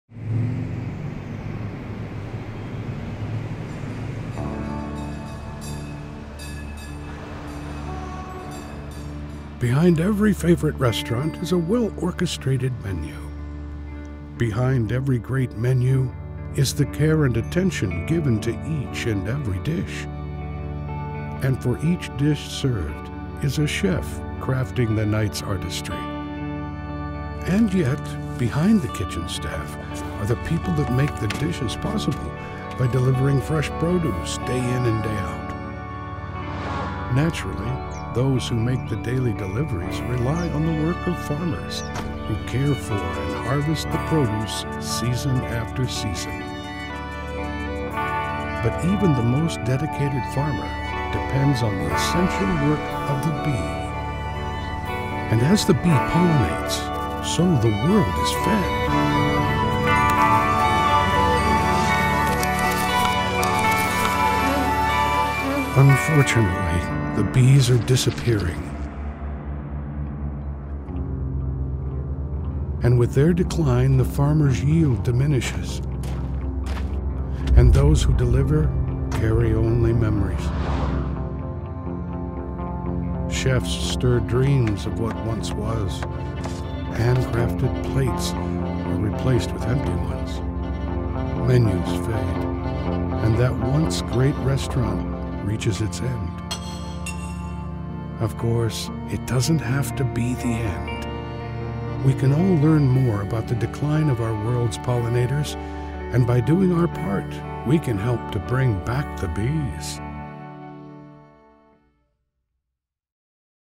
Give Bees A Chance l Share The Buzz l Whole Foods Market
GIVE BEES A CHANCE - The future of food depends on them.

Where would we be without Bees?
The lives of bees and humans are intricately linked.  Without bees, breakfast would be bland, lunch would be limited and dinner would be dismal.  In fact, one out of every three bites of food we eat is made possible by bees and other pollinators!